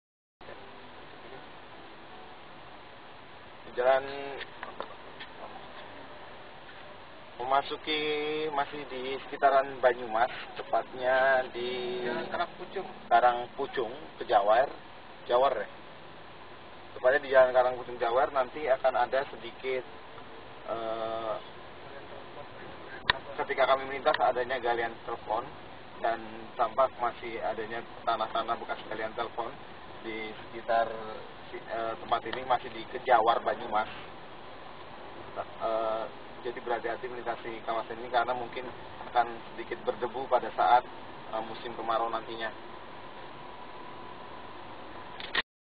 Kondisi jalan di jalan karang pucung, kejawar, banyumas, jateng
VIDEO HARI ke 3   pkl 10 10   Kondisi jalan di jalan karang pucung, kejawar, banyumas, jateng